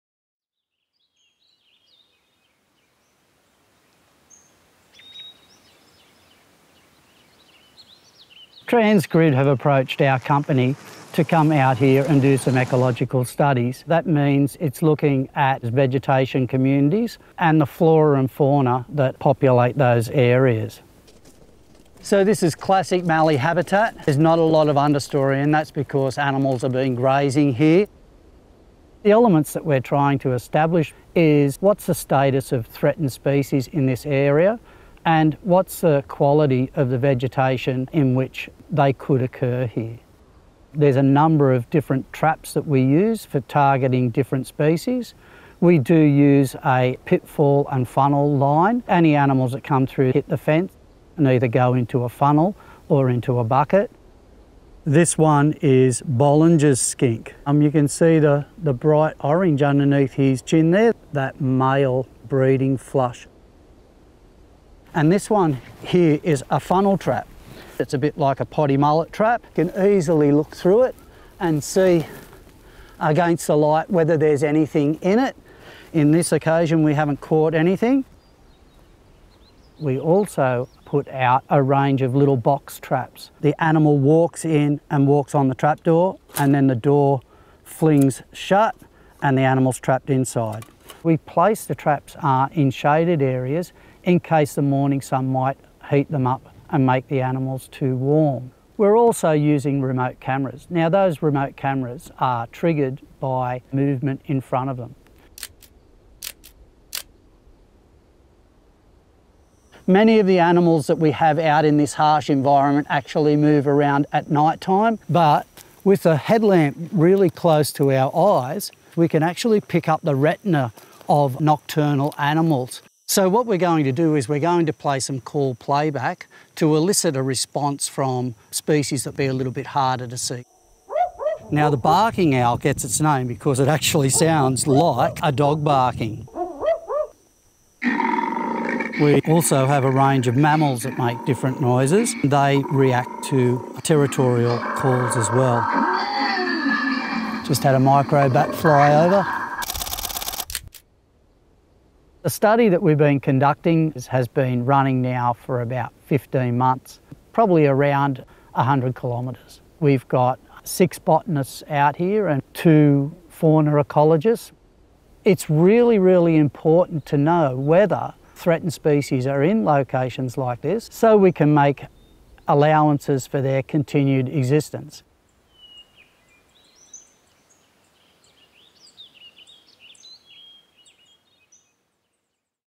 Transgrid EnergyConnect Ecological Survey
Transgrid conducts ecological surveys as part of its investigations and planning requirements for each major project. This ensures we have visibility of existing flora and fauna and habitats – information which forms part of the environmental impact statement required for each project.

We recently caught up with ecologists performing work in western NSW on the corridor for the proposed 900km interconnector EnergyConnect.